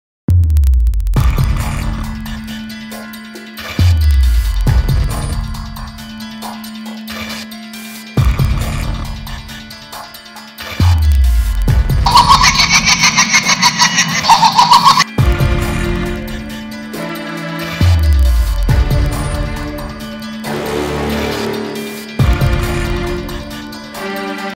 Saw Stop Motion
Just trying out stop motion here. Its my first attempt and just getting used to frame rate, music, lighting and etc.